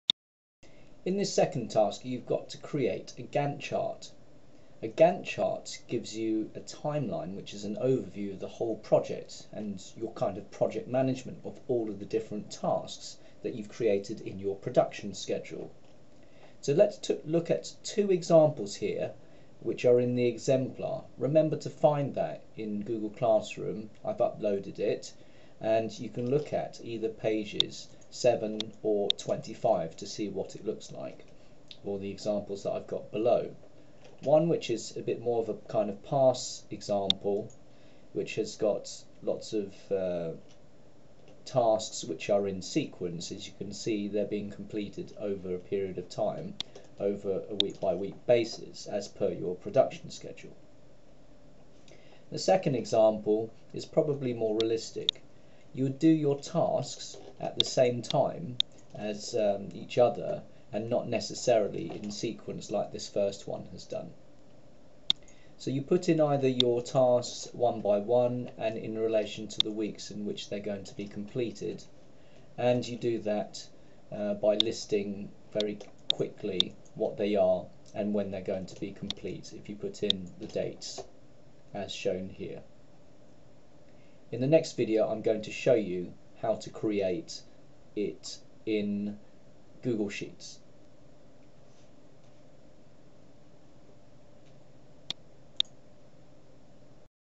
Lesson 05 P3 V2 GANTT Chart introduction
An explanation of what a GANTT is for a production.
It should link with a production schedule.